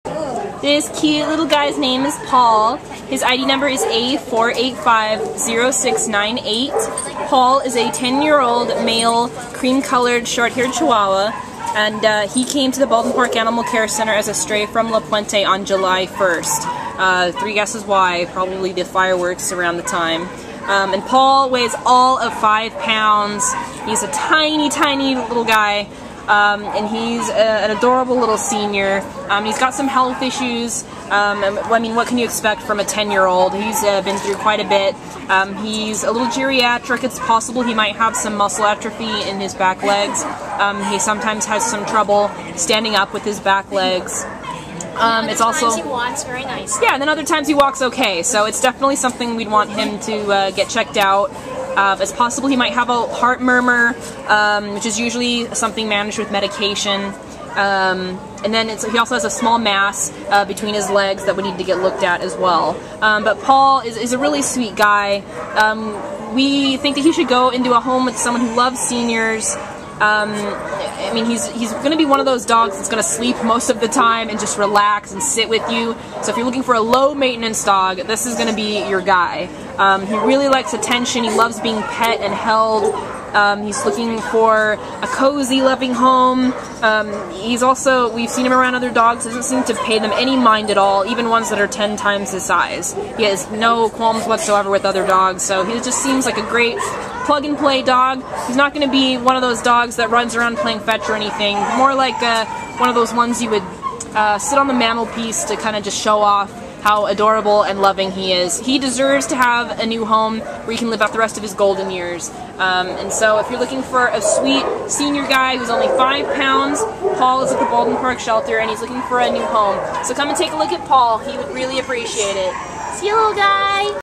A4850698 Paul | Chihuahua
A4850698 Paul is a soulful 10-year-old cream-and-tan male Chihuahua mix who was found as a stray in La Puente on July 1st and brought to the Baldwin Park Animal Care Center. Weighing 5 lbs, Paul is a typically calm and gentle senior dog who is looking for a cozy home where he can enjoy his golden years. He is happy to be carried or sit on your lap while you pet him. He does not appear to have any teeth, so he will likely need a diet of soft food, and he has a possible heart murmur and a small mass, so—as with all dogs, and especially seniors—a checkup by an outside veterinarian is a good idea. Still, Paul strikes us as having a lot of love left to give. He is so snuggly and kind, we can’t imagine that he will be anything other than a delightful addition to a quieter home that appreciates an older and loving dog.